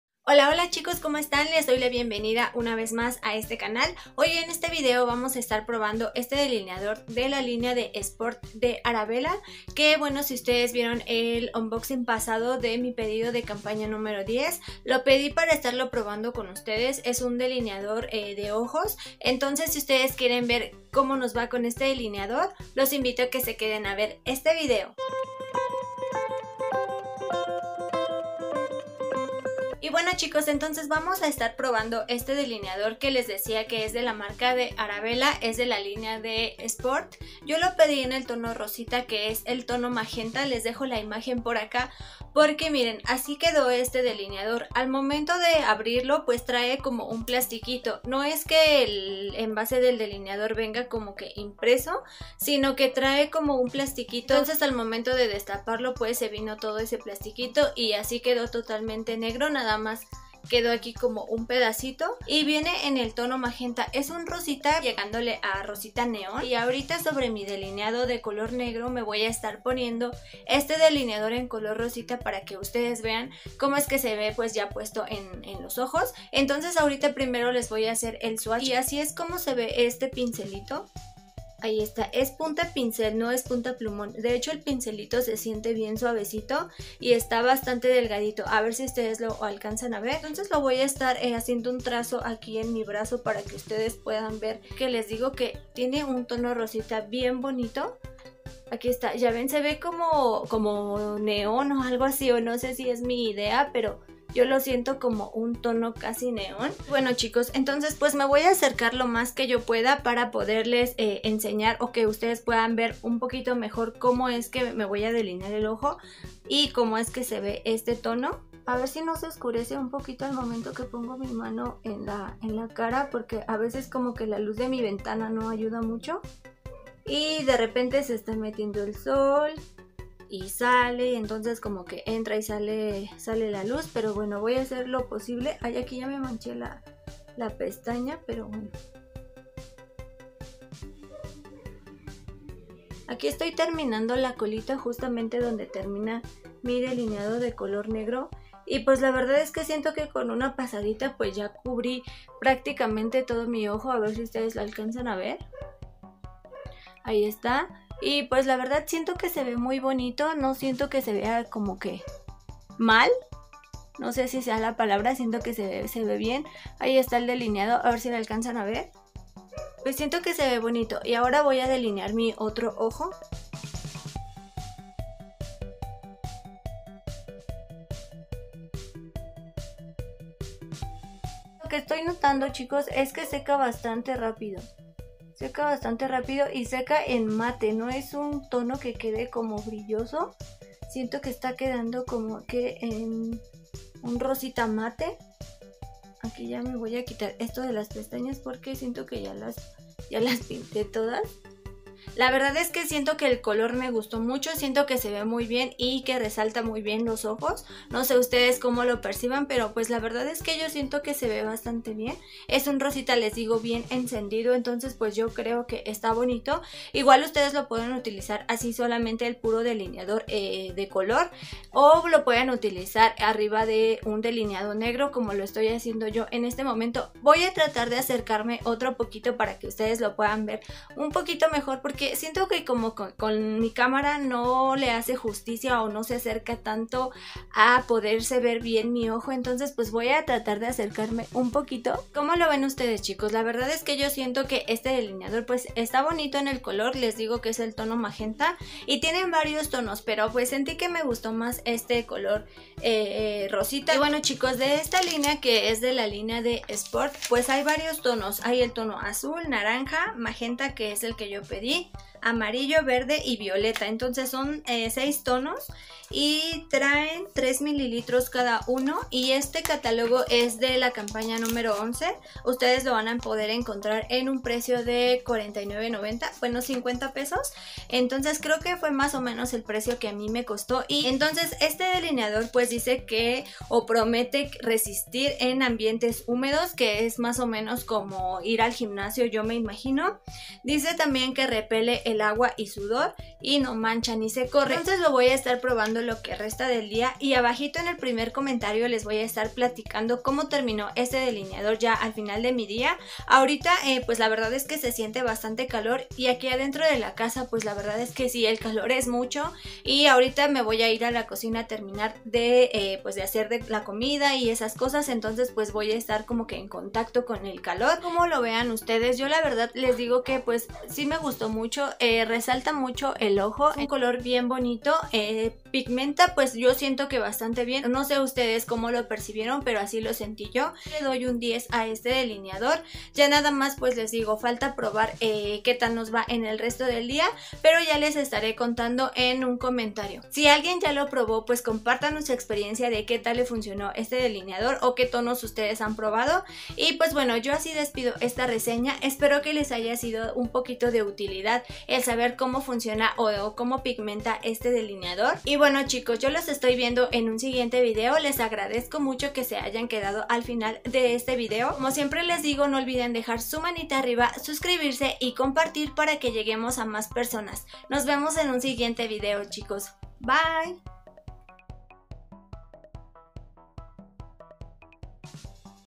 NUEVO DELINEADOR SPORT ARABELA
Hola chicos

Les comparto la reseña de éste delineador de Arabela Sport, me encantó el color 😃

Quieres comprar algún producto? Link a "Mi tiendita"